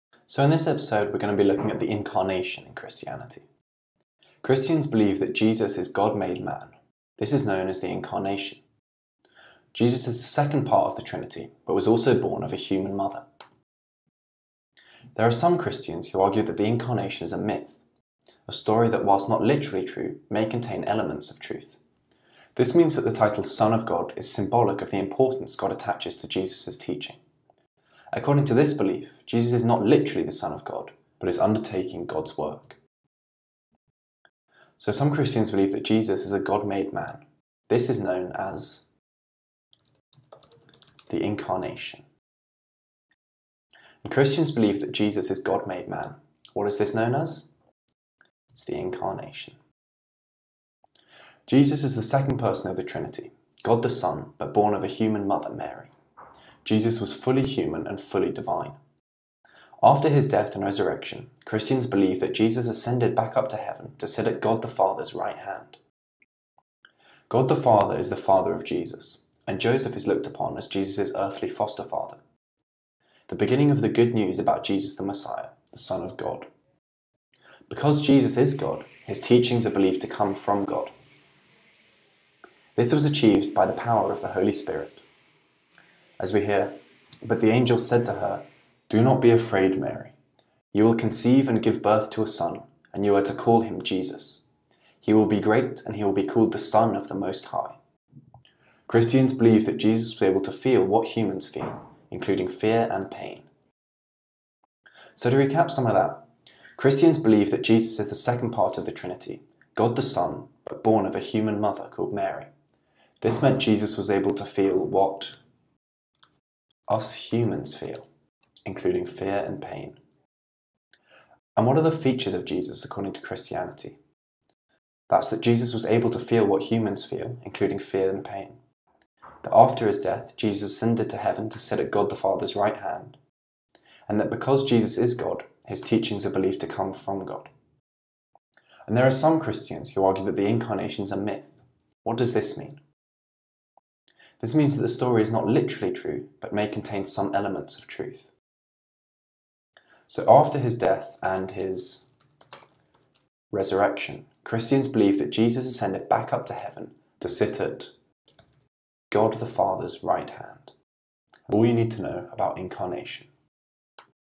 AQA GCSE Religious Studies Religions - Christianity - The Incarnation - Seneca - Learn 2x faster
Flinn looks at Christianity for your GCSE Religious Studies - Religions exam. In this episode, he will look at the Incarnation.